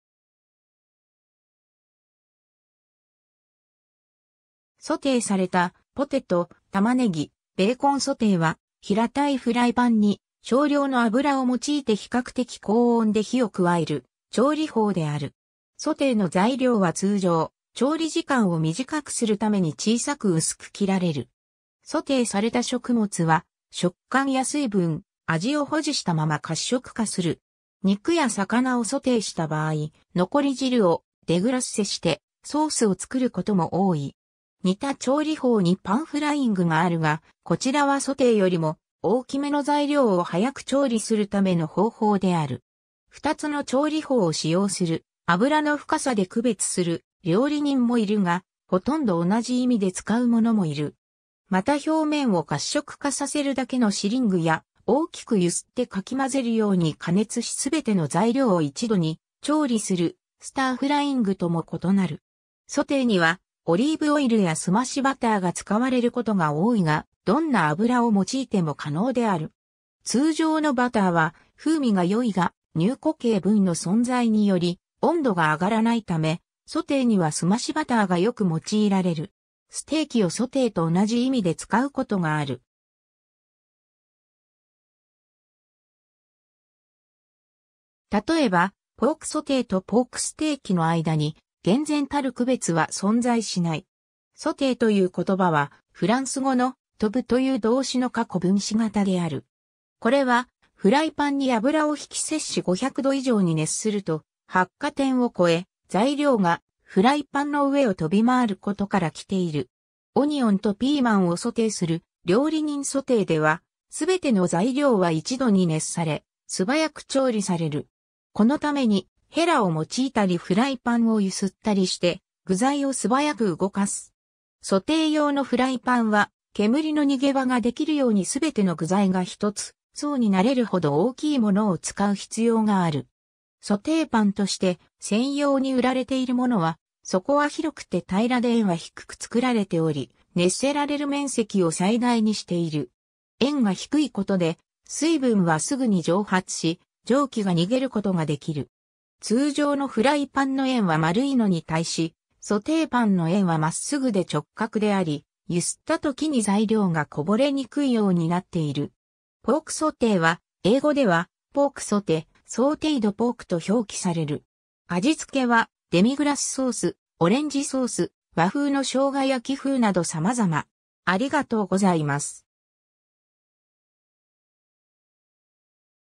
ソテー
ソテーされたポテト、玉ねぎ、ベーコン ソテー（仏: sauté）は、平たいフライパンに少量の油を用いて比較的高温で火を加える調理法である。
 ソテーの材料は通常、調理時間を短くするために小さく薄く切られる。
ソテーされた食物は、食感や水分、味を保持したまま褐色化する。
肉や魚をソテーした場合、残り汁をデグラッセしてソースを作ることも多い。
 似た調理法にpan fryingがあるが、こちらはソテーよりも大きめの材料を早く調理するための方法である。
2つの調理法を使用する油の深さで区別する料理人もいるが、ほとんど同じ意味で使う者もいる。
また表面を褐色化させるだけのsearingや、大きく揺すってかき混ぜるように加熱し全ての材料を一度に調理するstir fryingとも異なる。
 ソテーにはオリーブオイルや澄ましバターが使われることが多いが、どんな油を用いても可能である。
通常のバターは風味が良いが、乳固形分の存在により温度が上がらないため、ソテーには澄ましバターが良く用いられる。
ステーキをソテーと同じ意味で使うことがある。
例えば、ポークソテーとポークステーキの間に厳然たる区別は存在しない。
 ソテーという言葉は、フランス語の「跳ぶ」という動詞の過去分詞型である。
これは、フライパンに油を引き500℃以上に熱すると、発火点を超え、材料がフライパンの上を跳び回ることから来ている。
 オニオンとピーマンをソテーする料理人 ソテーでは、全ての材料は一度に熱され、素早く調理される。
このために、へらを用いたりフライパンを揺すったりして、具材を素早く動かす。
 ソテー用のフライパンは、煙の逃げ場ができるように全ての具材が一つ層になれるほど大きいものを使う必要がある。
ソテーパンとして専用に売られているものは、底は広くて平らで縁は低く作られており、熱せられる面積を最大にしている。
縁が低いことで、水分はすぐに蒸発し、蒸気が逃げることができる。
通常のフライパンの縁は丸いのに対し、ソテーパンの縁は真っ直ぐで直角であり、揺すった時に材料がこぼれにくいようになっている。
 ポークソテーは、英語では「pork sauté」「sautéed pork」と表記される。
味付けはデミグラスソース、オレンジソース、和風の生姜焼き風など様々。